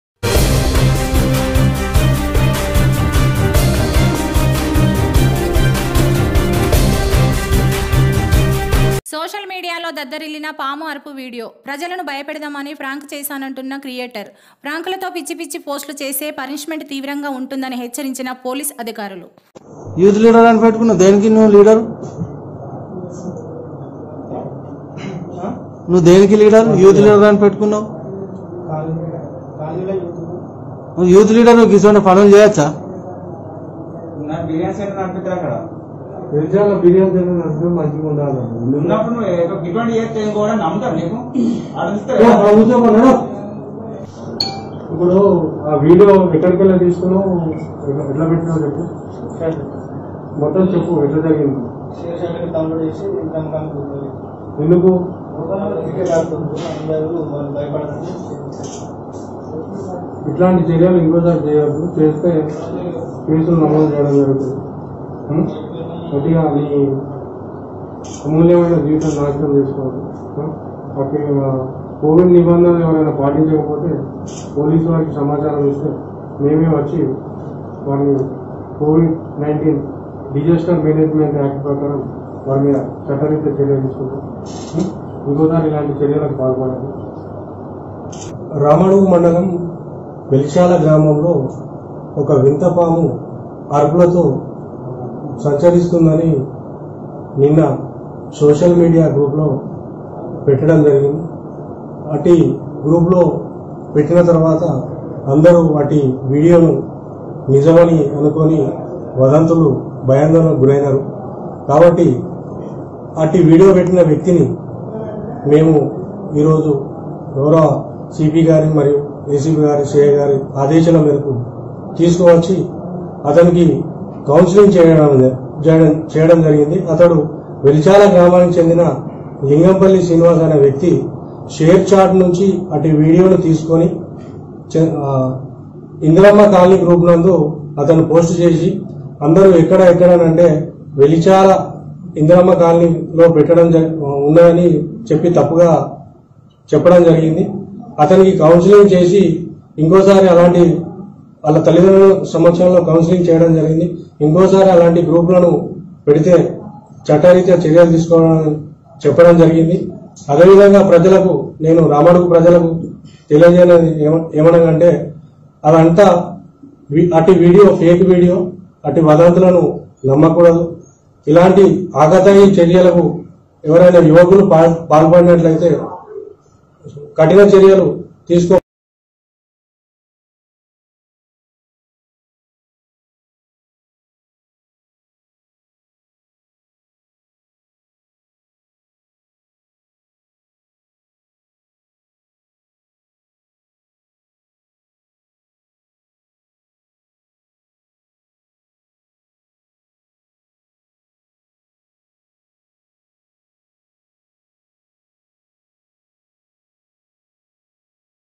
ప్రాంక్ వీడియో పోస్టులతో అసత్యాలను ప్రచారం చేస్తే తగు చర్యలు ఉంటాయన్నా పోలీస్ అధికారులు
సోషల్ మీడియాలో దద్దరిళ్లిన పాము అరుపు వీడియో,
ప్రజలను బయపెడదామని ఫ్రాంక్ చేశానంటున్న క్రియేటర్...ఫ్రాంకులతో పిచ్చిపిచ్చి పోస్ట్లు చేస్తే పనిష్మెంట్ తీవ్రంగా ఉంటుందని హెచ్చరించిన పోలీస్ అధికారులు.